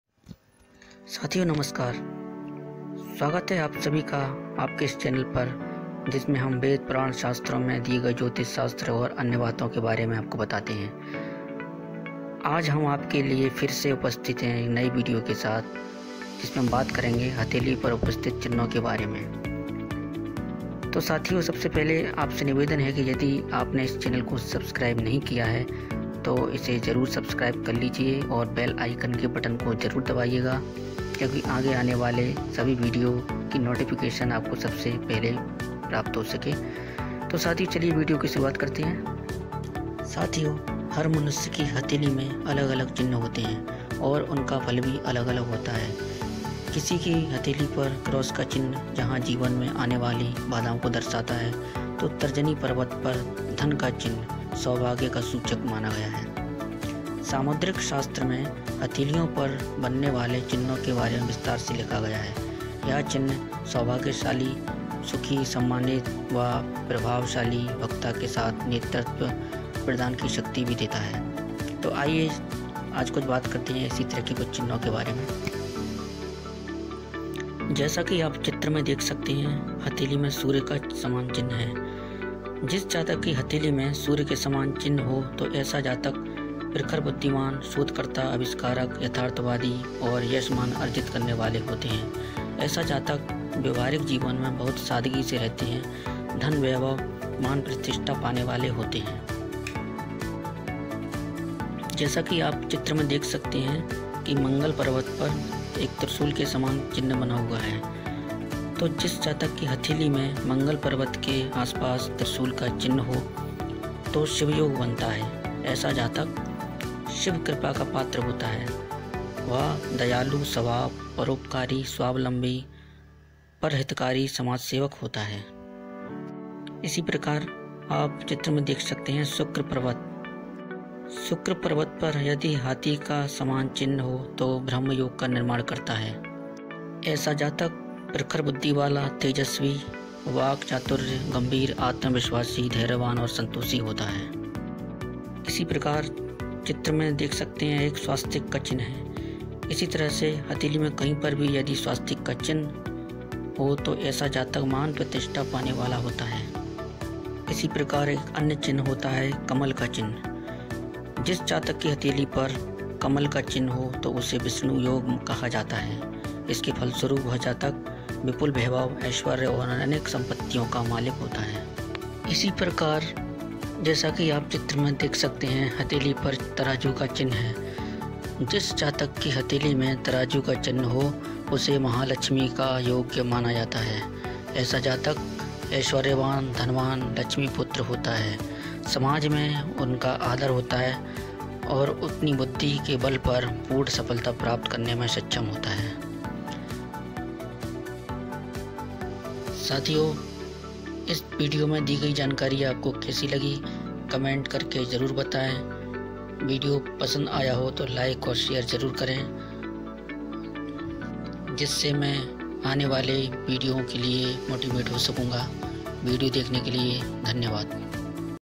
Palm reading | Palmistry
क्या कहते हैं आपके हथेली के चिन्ह | how to learn astrology

इस वीडियो में हम आप को बताएँगे कि किस तरह से आप अपने हाथ की रेखाओं को देखकर खुद के भविष्य के बारे मे जानकारी प्राप्त कर सकते हैं